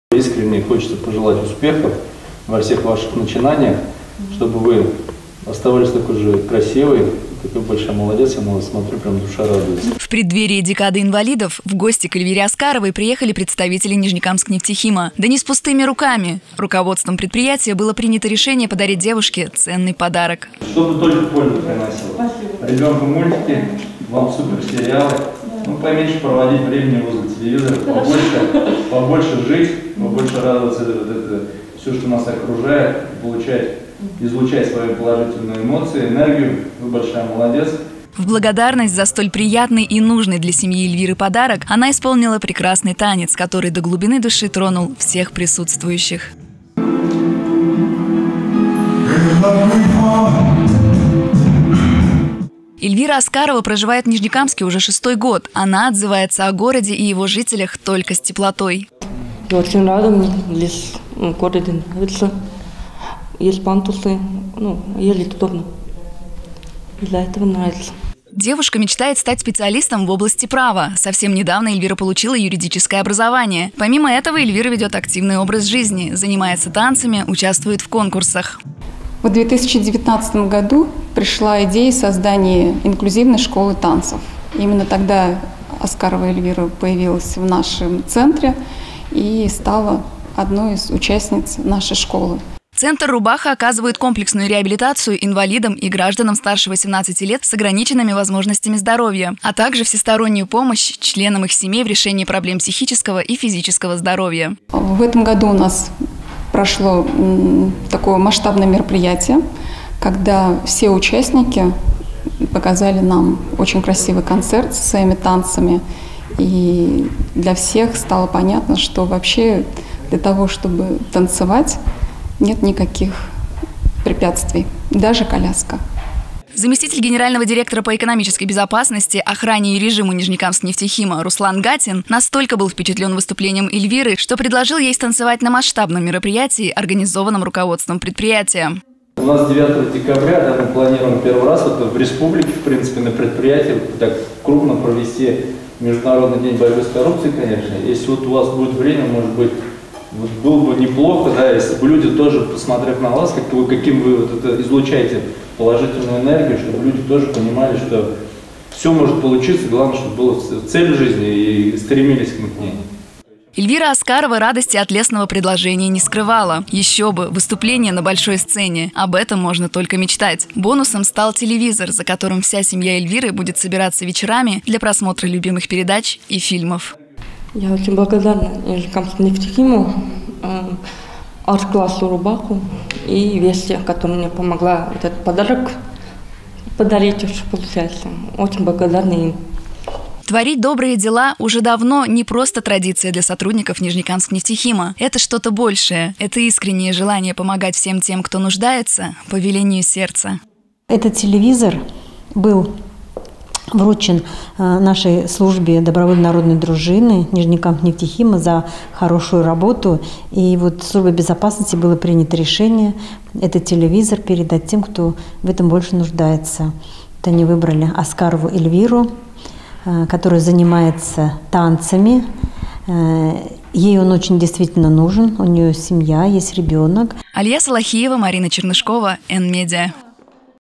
ДОБРАЯ ПОМОЩЬ ОТ НКНХ
За последние годы волонтерское движение «Нижнекамскнефтехима» окрепло и приросло новыми направлениями работы.

Благотворительная помощь медицинским учреждениям в период самоизоляции была оказана в помощь ветеранам Великой Отечественной войны и труженикам тыла, инвалидам первой и второй группы, оказывается постоянная поддержка подшефным школам, дошкольным образовательным учреждениям и отдельным людям.

Так, соблюдая традиции добрых дел, представители предприятия вручили ценный подарок Эльвире Аскаровой - девушке с ограниченными возможностями здоровья.